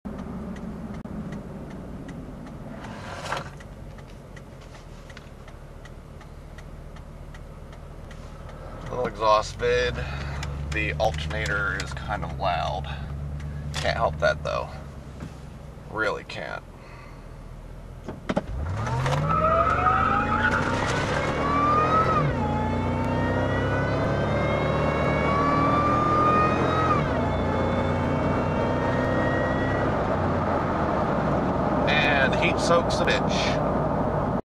Short in car of my truck.
This was taken as a joke as an exhaust vid.  Ignore the driver commentary...As far as the heat-soak goes, my Intercooler temp was 112 degrees after the vid was taken.  Ambient temp was 77.  several runs were made to try and get a clear vid, hence the high IC temps.